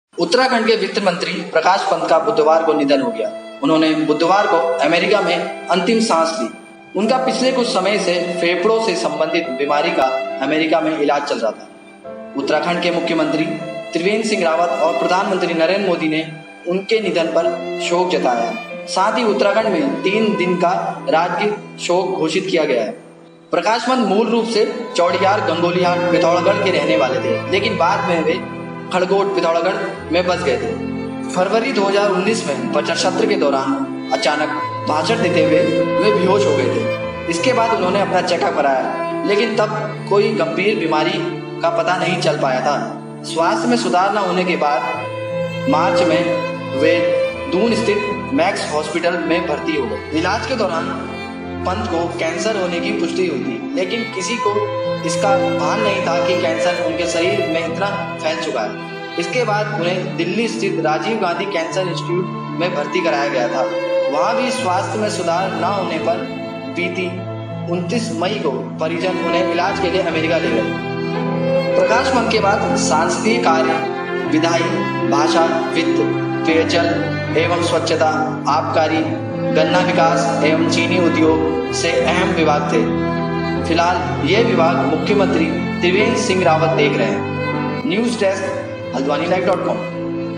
Uttarakhand finance minister Prakash Pant passes away in the US.
उत्तराखण्ड के वित्तमंत्री प्रकाश पंत का अमेरिका में निधन हो गया। वो लंबे वक्त से बीमार थे।  Uttarakhand finance minister Prakash Pant passes away in the US